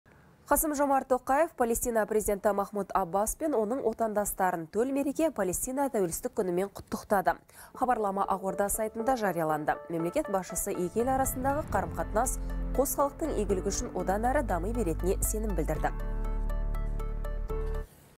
Қ.Тоқаев Палестина президентіне құттықтау жеделхатын жолдады
Қасым-Жомарт Тоқаев Палестина Президенті Махмуд Аббас пен оның отандастарын төл мереке – Палестина Тәуелсіздік күнімен құттықтады.